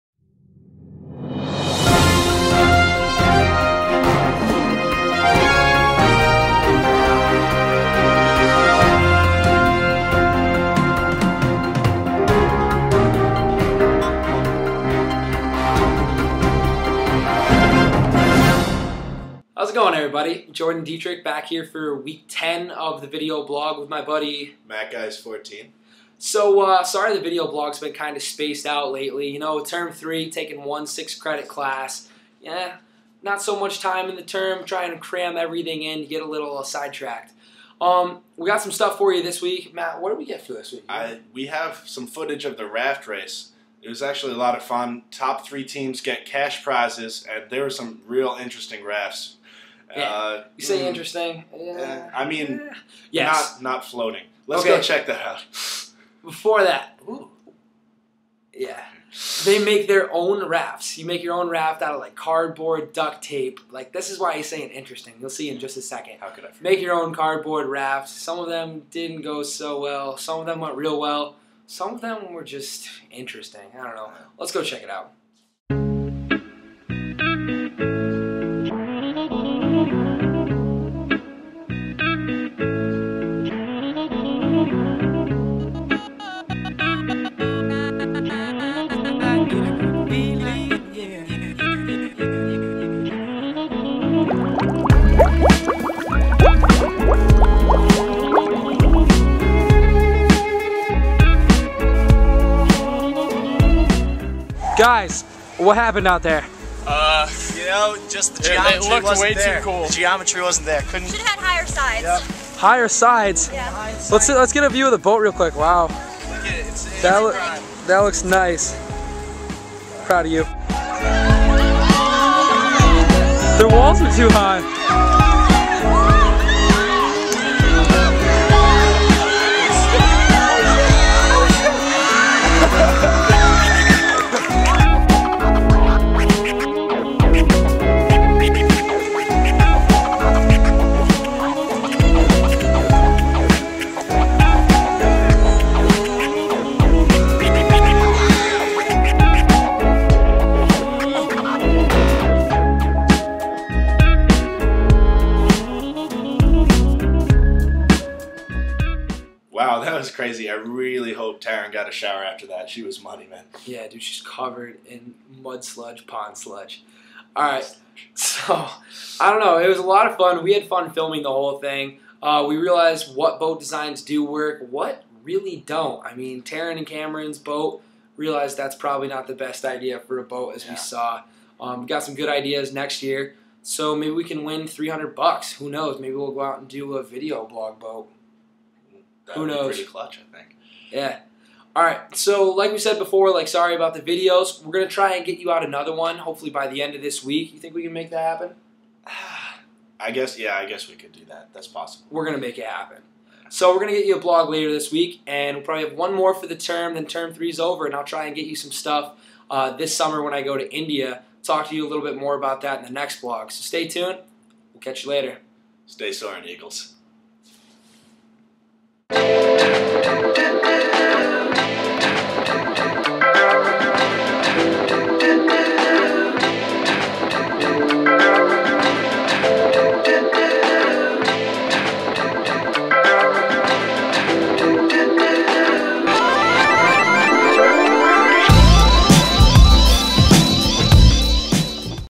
Elmira College Student Video Blog #10
This week check out footage of the Term 3 Radical Raft race, with Matt and Jordan.